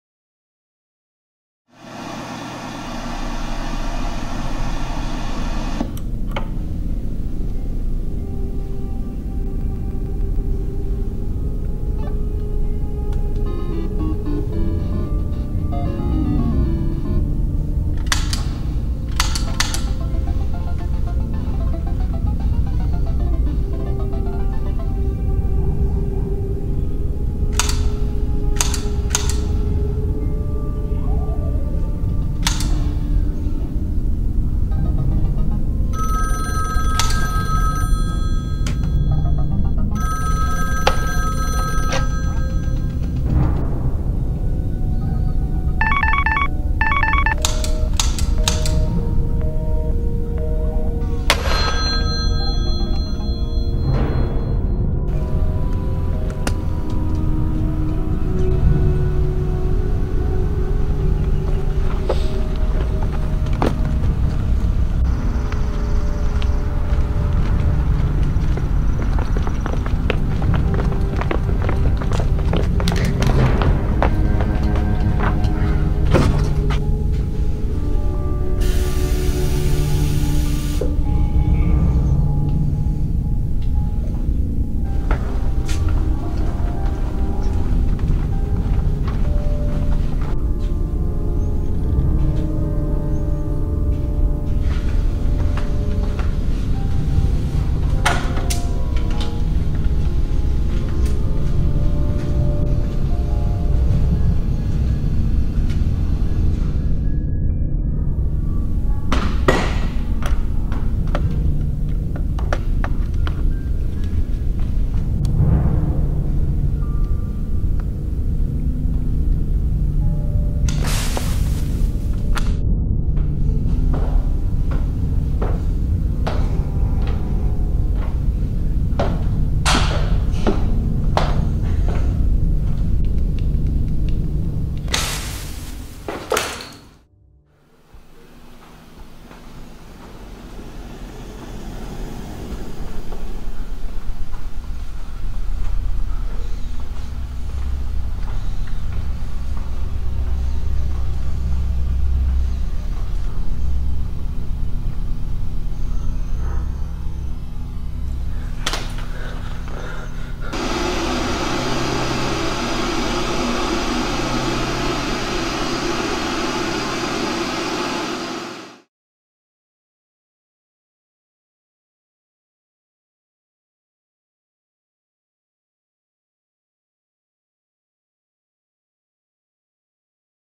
A Shot in the Dark - Ryan Thomas
A Tv Class video project where no one could talk, or have dialogue. I believe it worked quite well and is a very solid video.